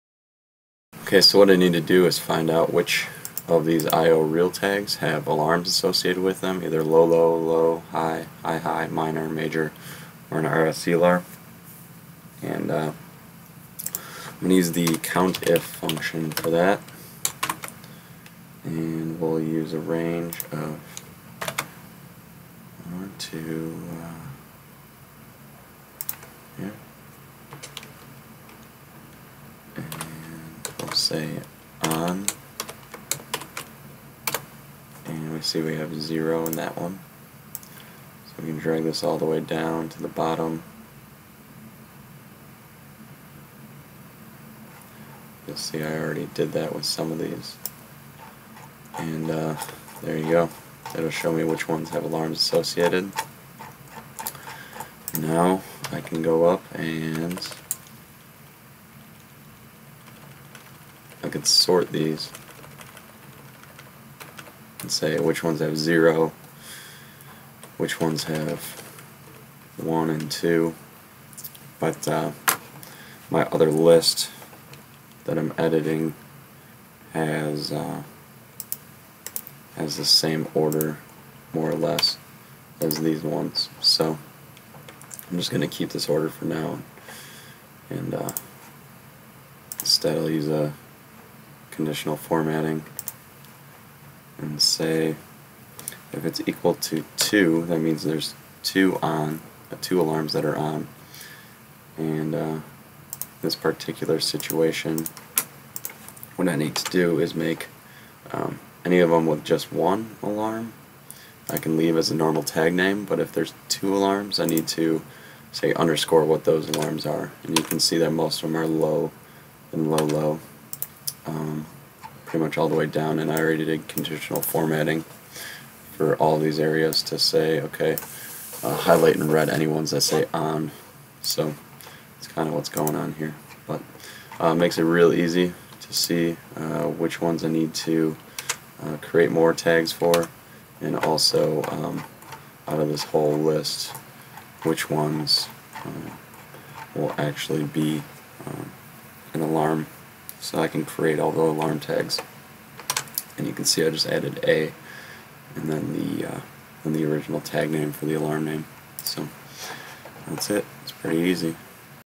Wonderware - Sorting Tags with Excel
We take a look at a huge list of tags and find which ones have alarms associated with them using Excel COUNTIF and conditional formatting